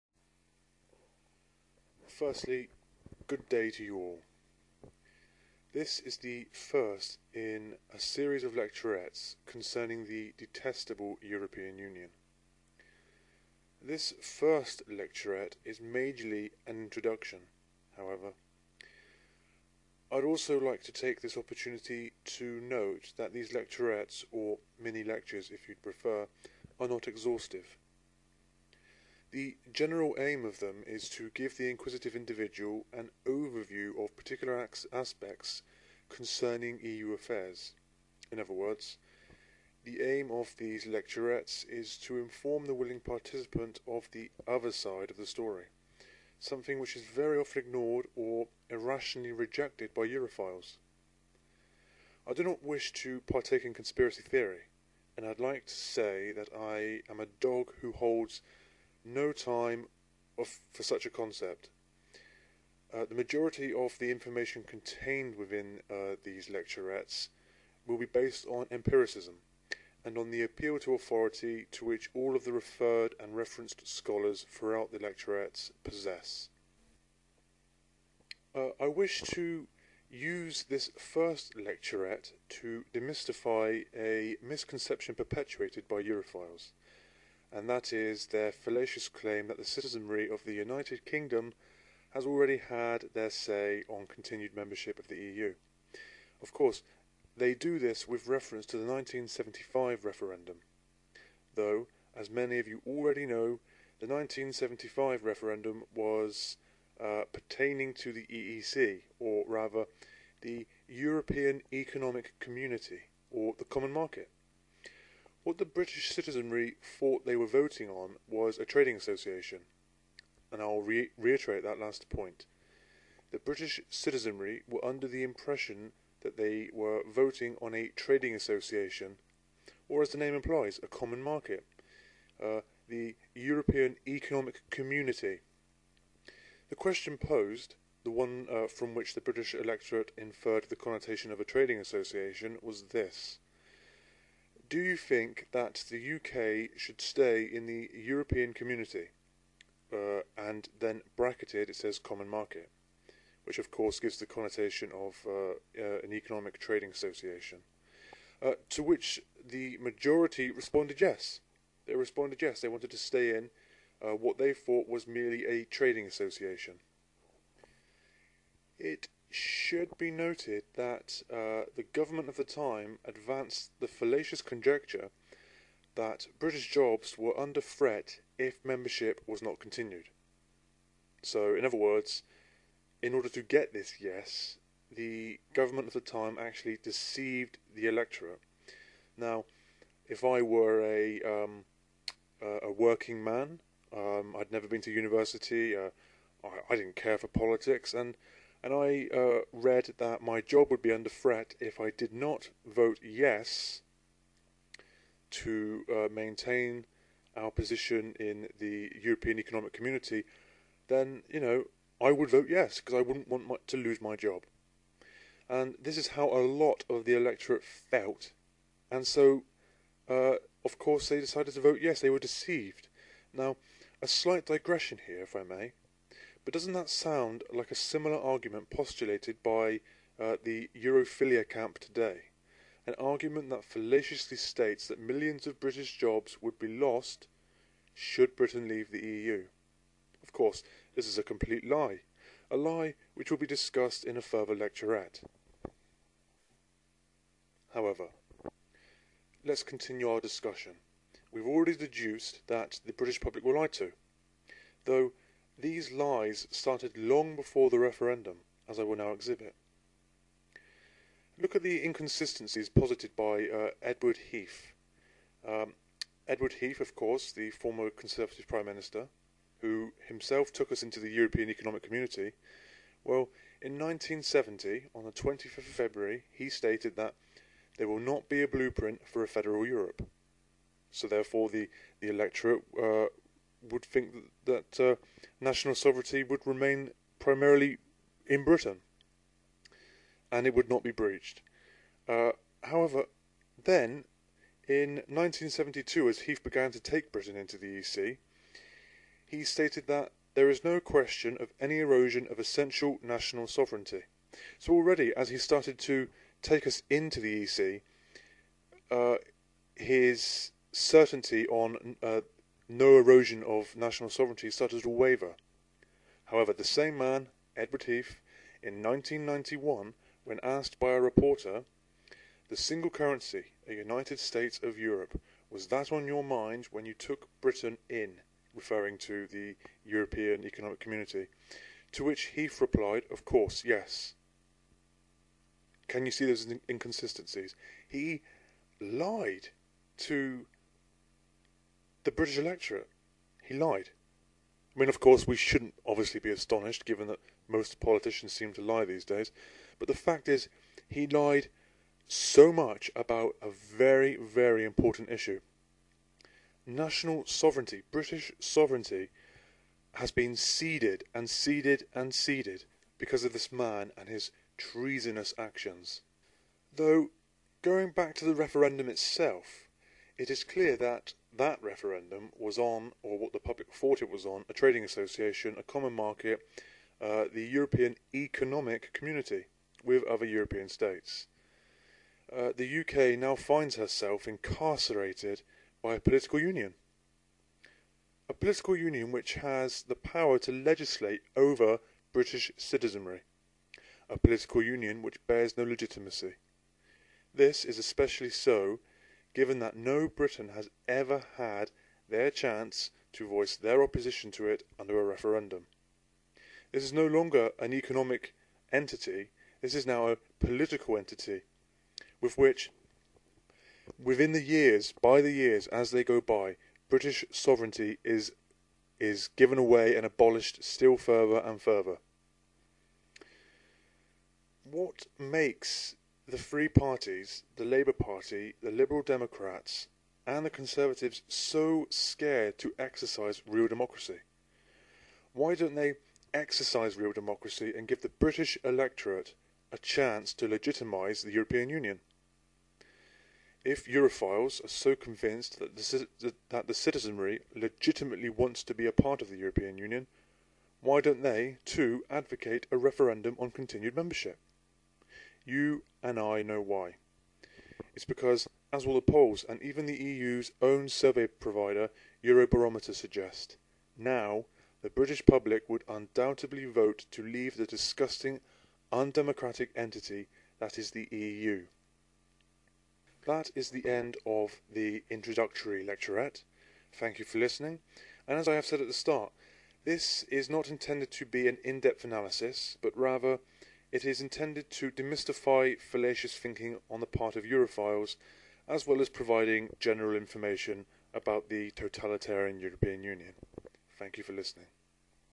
Talking Dog: Lecturette One - Introduction and the 1975 Referendum (EU affairs)
This is a brief introduction to a new series of lecturettes (mini-lectures) relating to the corrupt totalitarian EU. It includes a mini-discussion on the misconception postulated by Europhiles that the UK has had its say on being in the EU and voted in favour of it.